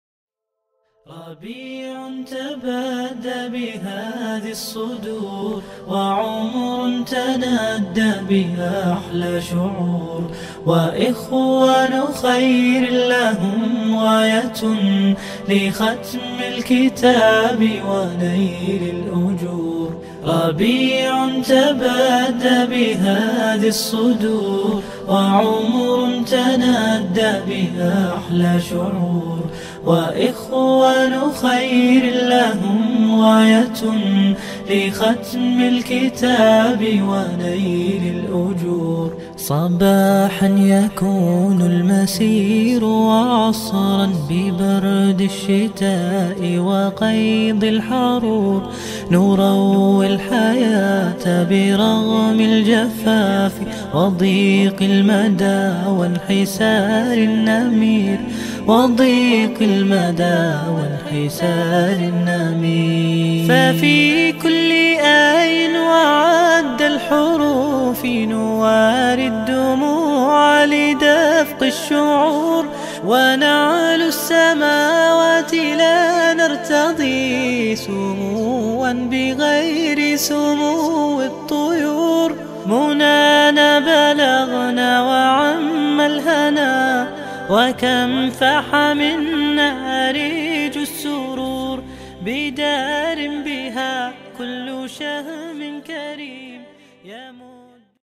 حفل الختم القرآني الزاوية العابدية 21 جانفي 2022
قل بفضل الله و برحمته فبذلك فليفرحوا
جمعية العلماء المسلمين "شعبة الزاوية العابدية" بولاية تقرت، تزف للجزائر دفعتها الثانية  المكونة من :
🔸️27 حافظا وحافظة لكتاب الله تعالى
🔸️40 متقنا لتلاوة القرآن الكريم